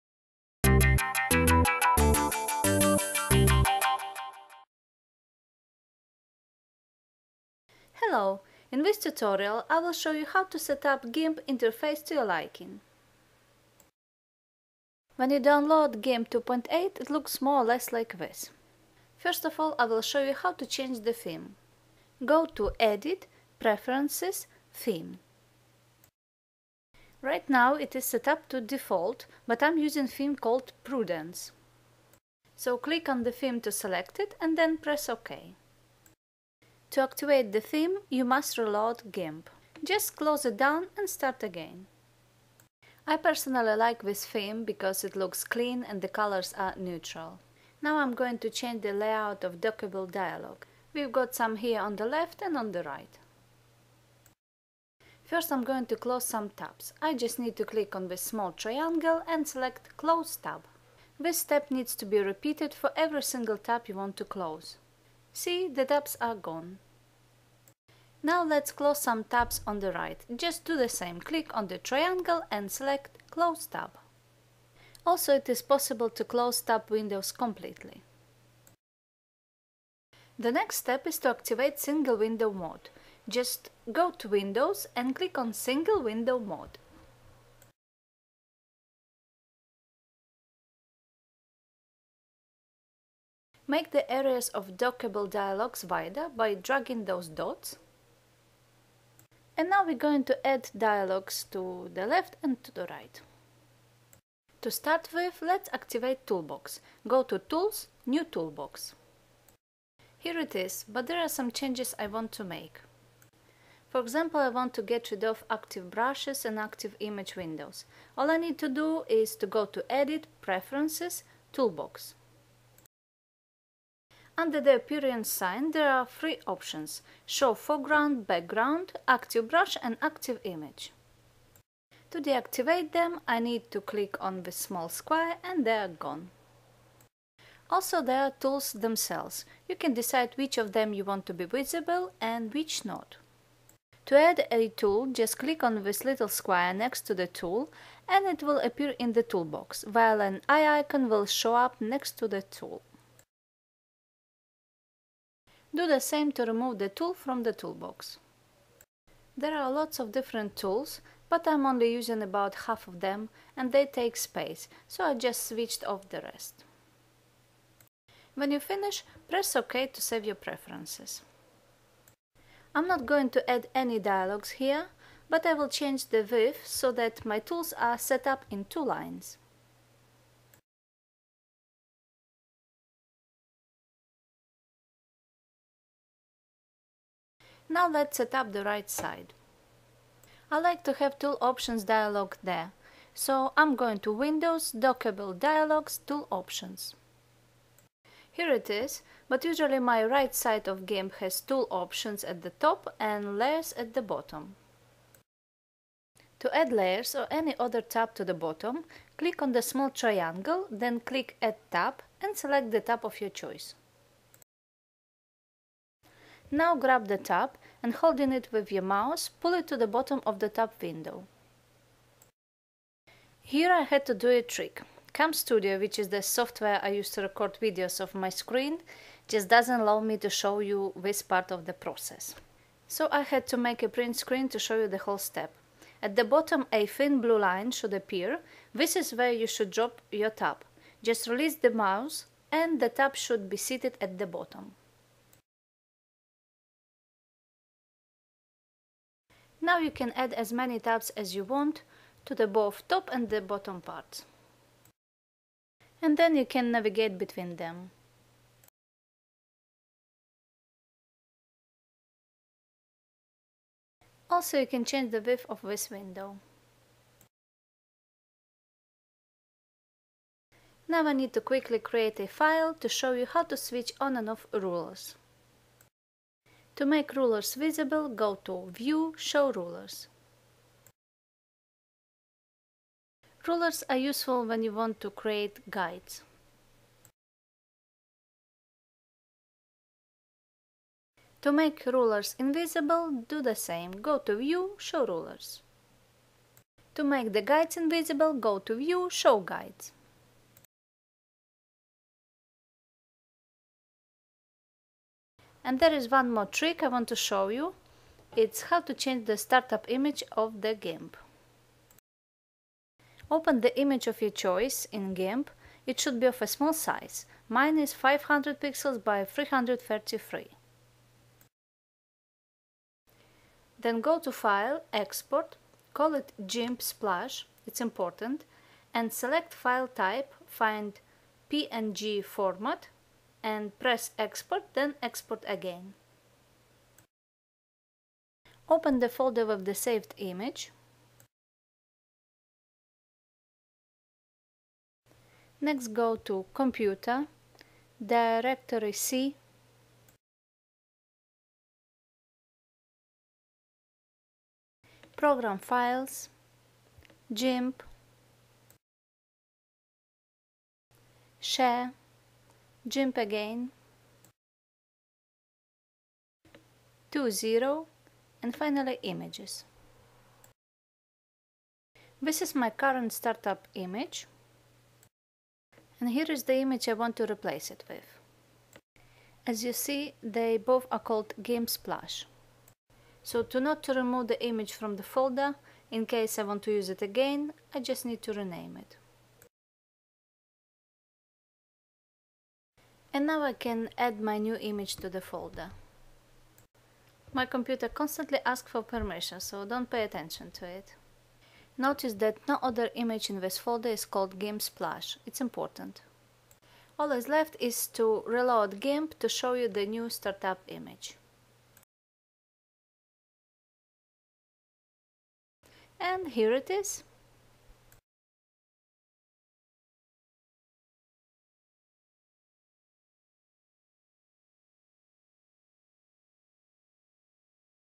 GIMP 2.8 Interface: Theme, Tabs, Startup image
In this video I will show you how I set up the GIMP Interface the way you see it in my videos.

Directory to change startup image: Computer / C / Program Files / Gimp / Share / Gimp / 2.0 / Images

GIMP is a free software and it can be downloaded